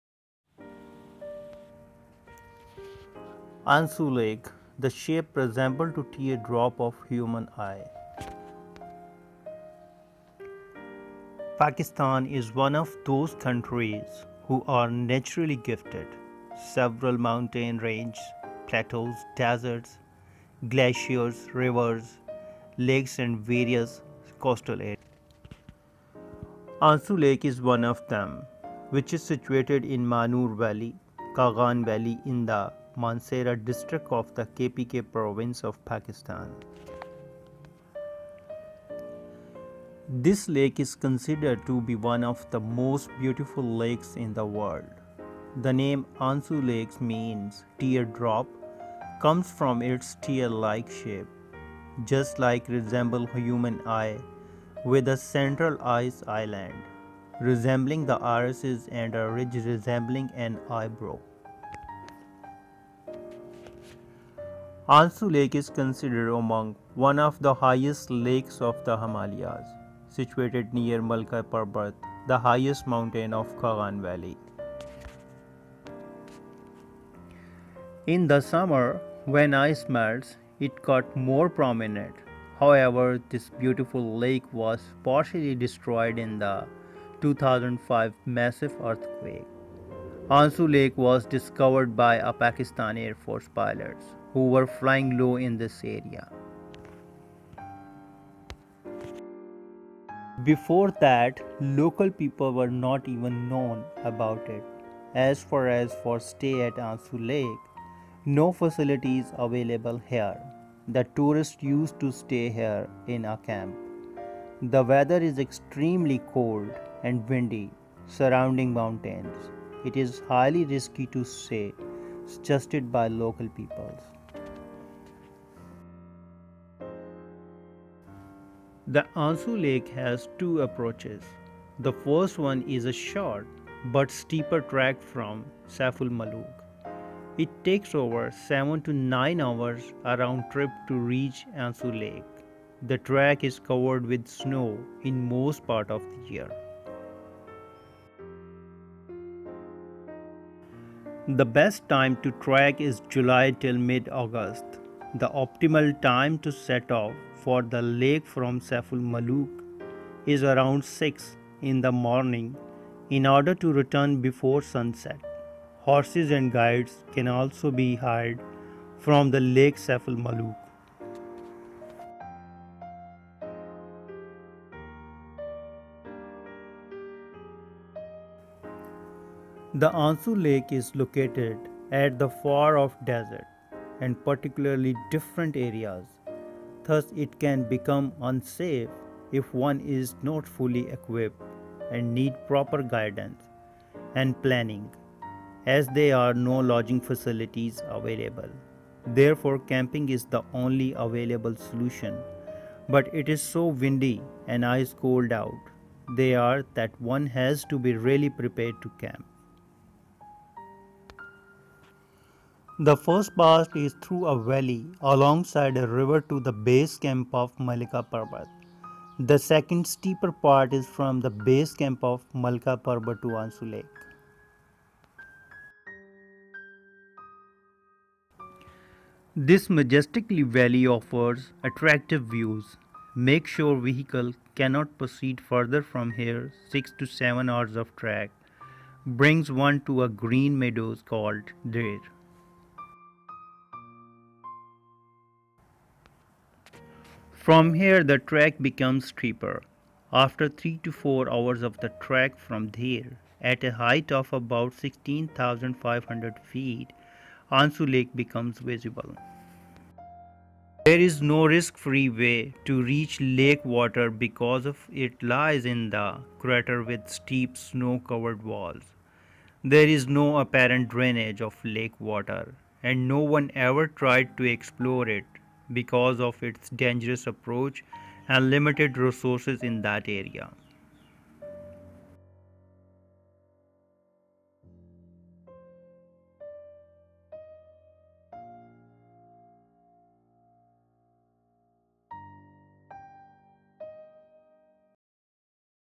Ansoo Lake – The Shape Resembles to Teardrop of Human Eye
Pakistan is one of those countries, who are naturally gifted, several mountain ranges, plateaus, deserts, glaciers, rivers, lakes, and various coastal areas. Ansoo Lake is one of them, which is situated in Manoor Valley, Kaghan Valley in the Mansehra District of the KPK province of Pakistan.
This lake is considered to be one of the most beautiful lakes in the world. The name Ansoo means teardrop, comes from it’s tear-like shape, just like resembles human eye with a central ice island resembling the iris and a ridge resembling an eyebrow. Ansoo Lake is considered among one of the highest lakes of the Himalayas, situated near Malika Parbat, the highest mountain of Kaghan Valley.